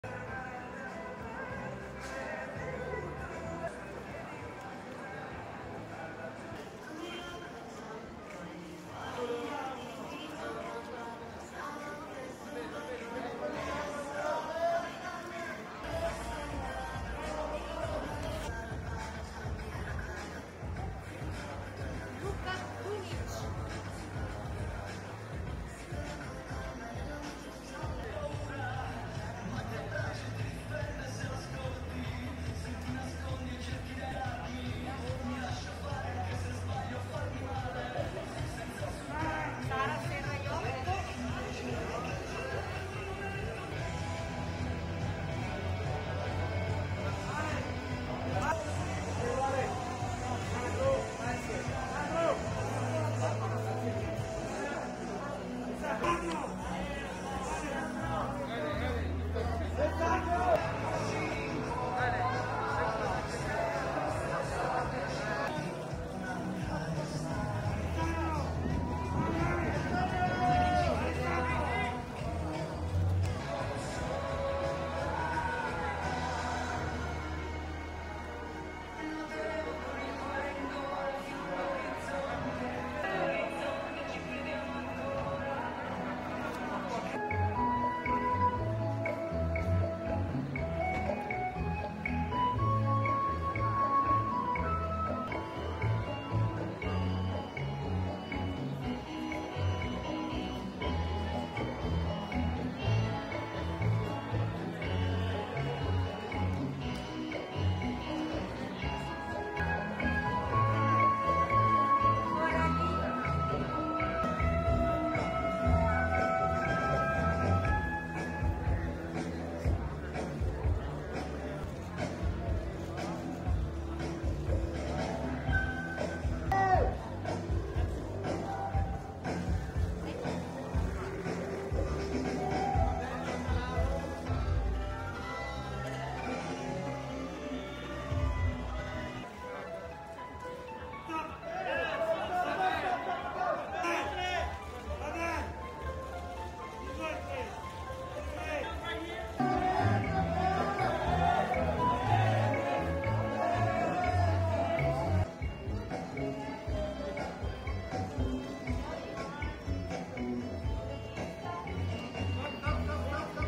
Adele Exarchopoulos and more walk the red carpet at the 77th Venice Film Festival
Sara Serraiocco, Sabrina Knaflitz and Alessandro Gassmann, Quentin Dupieux, David Marsais, Gregoire Ludig, Adele Exarchopoulos and more walk the red carpet at the 77th Venice Film Festival

Venice, Italy on Saturday, September 5, 2020